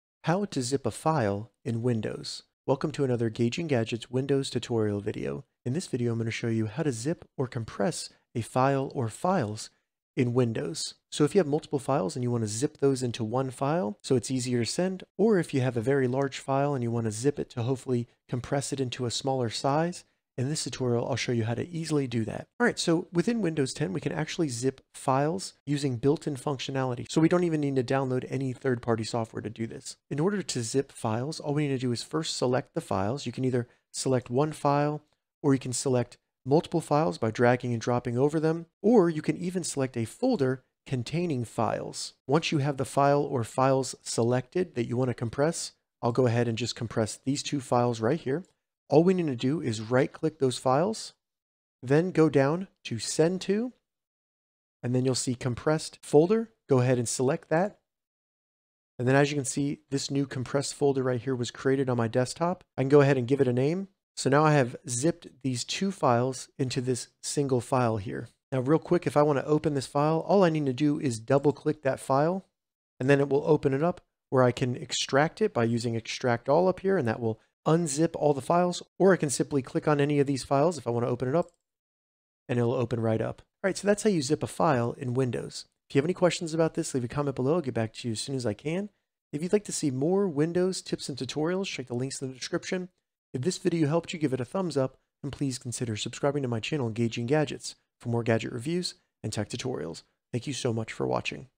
How to ZIP a File in Windows - Make ZIP Files in Windows 10 or 11
How to Zip a file in Windows? In this tutorial, I show you how to zip or compress any file or folder on your Windows 10 or Windows 11 PC. This means you can take many files and zip them into one file that is usually smaller in size.

Follow the steps below to zip files or folders on Windows 10:
1. Find and select the files on your Windows 10 or Windows 11 computer that you'd like to zip or compress.
2. Right click the file.
3. In the menu, select Send to, then select Compressed (zipped) folder.
4. Name the new zipped folder.
5. You have now zipped a file or folder.